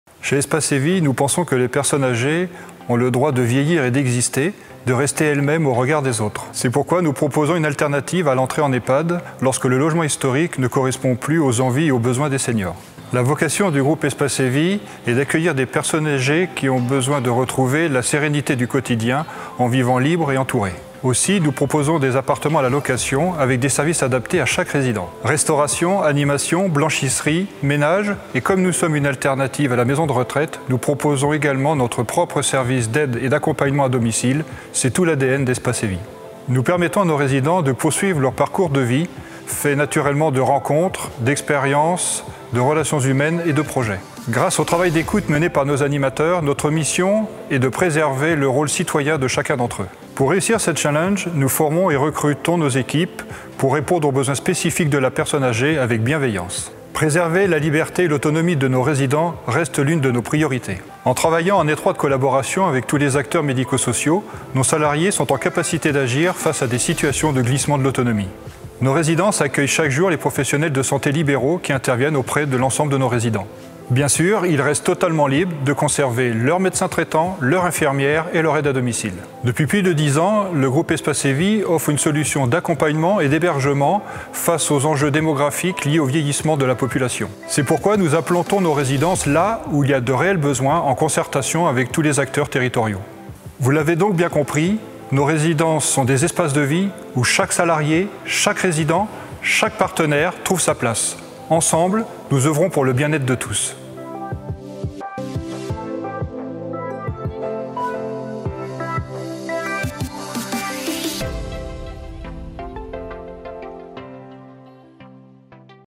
Les engagements des résidences seniors Espace & Vie par Laurent Guillet
Apprenez-en plus sur les engagements qui animent le groupe et les résidences services seniors Espace & Vie au quotidien.

Laurent Guillet, Président du Groupe vous explique le fonctionnement et les valeurs d'Espace & Vie.

Espace & Vie : La liberté préservée, la sérénité retrouvée